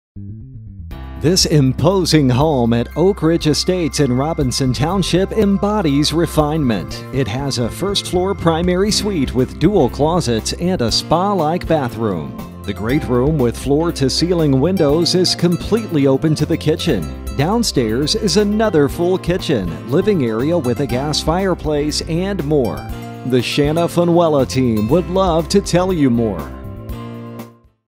219 August Drive Coraopolis, PA | ColdwellBankerHomes.com
219 August Drive, Coraopolis, PA

This imposing home at Oak Ridge Estates in Robinson Township embodies refinement. You are welcomed into the great room with its impressive floor-to-ceiling windows through the ample foyer, which also leads to the designated office area to the right and the main-floor primary bedroom to the left. The